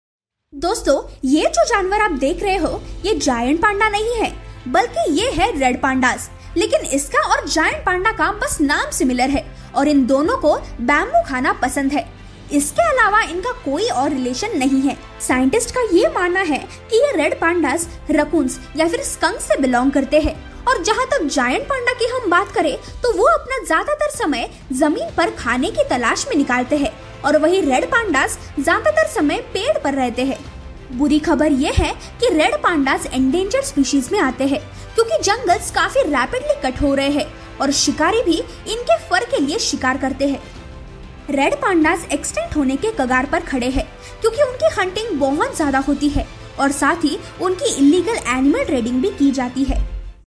Amazing Facts About Red Panda | FactStar
Red pandas and giant pandas share a similar name—and a love for bamboo—but they aren’t closely related. Scientists think that red pandas are more closely related to weasels, raccoons, and skunks. And while giant pandas spend most of their time roaming around on the ground looking for food, red pandas spend about 90 percent of their time in the trees in the mountains of Nepal, Myanmar, and central China where they live.

Red pandas are endangered. Their forest homes are being cut down, and poachers hunt them for their fur. And their adorableness might put them in more danger, since they’re ideal targets for the illegal pet trade

Red pandas have different names depending on where you are. In Nepal, they're called bhalu biralo.

Video Team :
Video Editor - Omkar Amble
creator - Nishidha bagayatkar